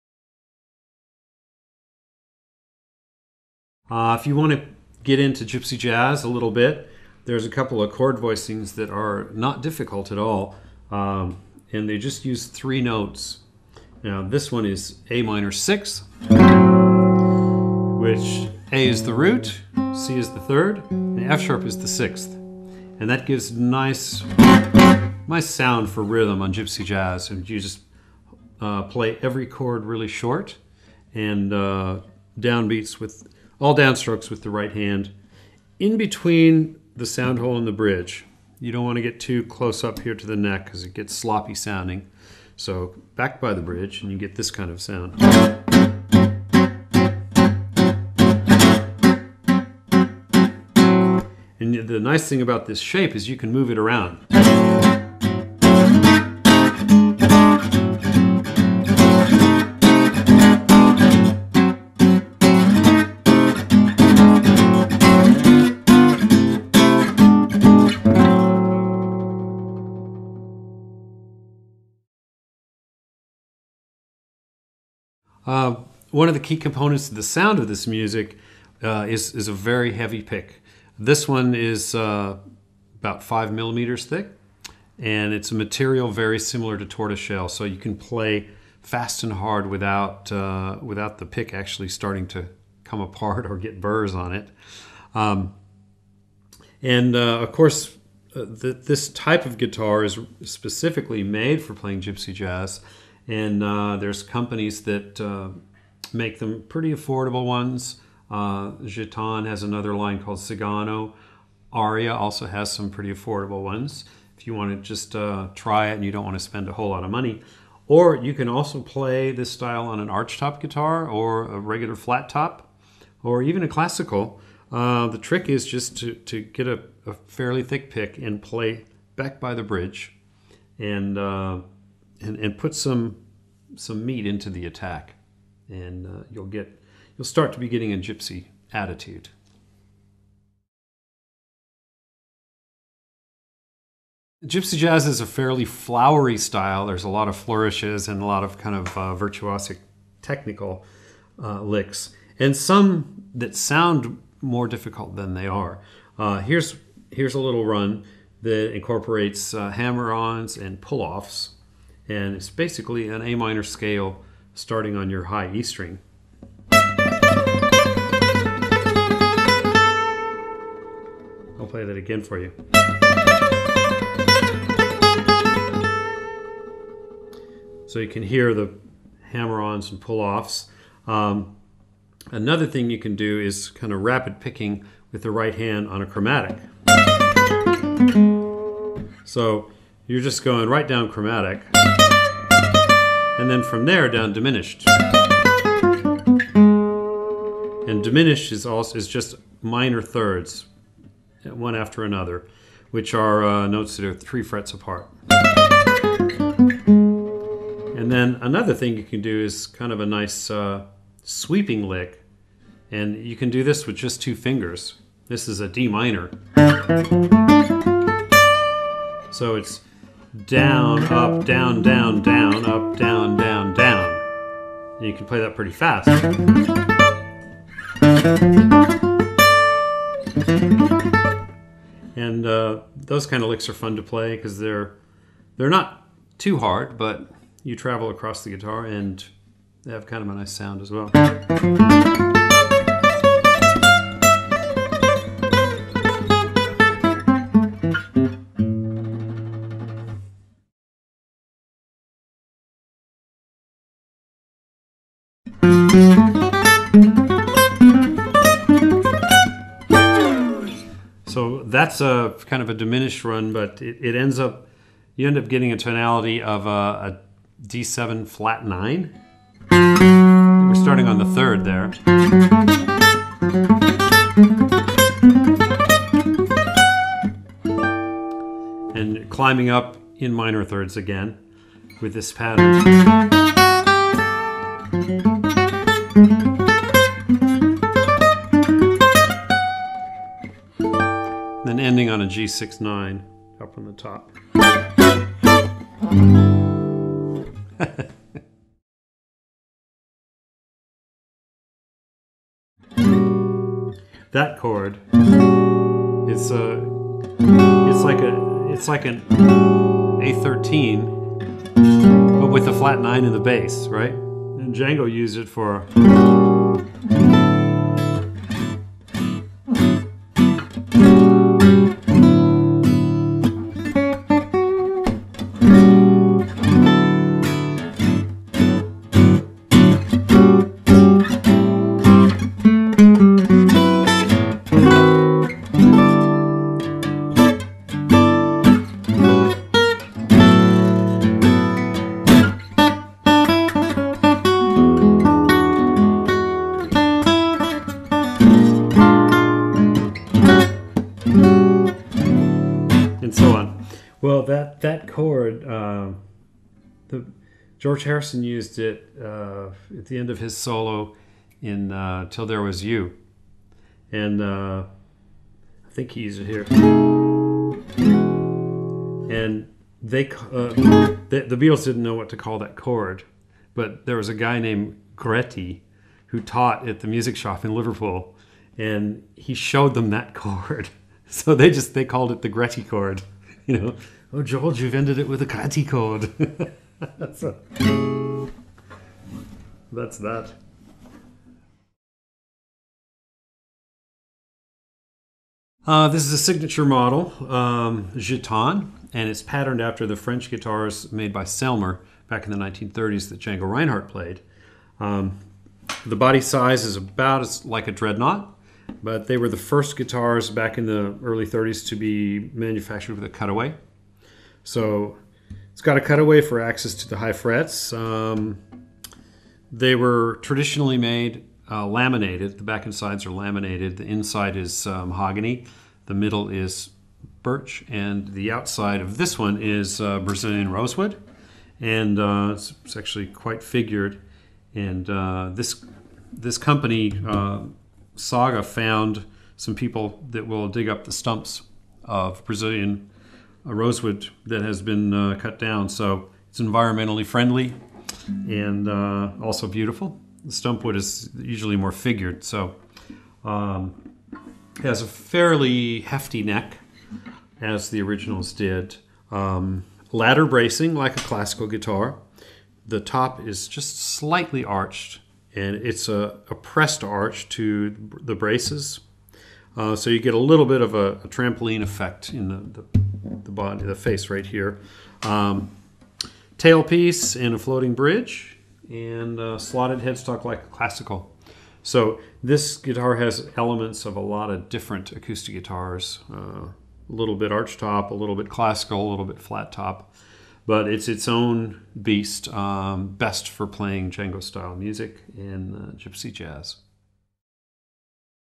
John Jorgenson Guide to Gypsy video tutorial Guitarist Magazine
An exclusive video lesson from one of the most accomplished modern players of the genre. Taken from Guitarist 319. For more guitar news, advice and interviews, and to find out more about the magazine,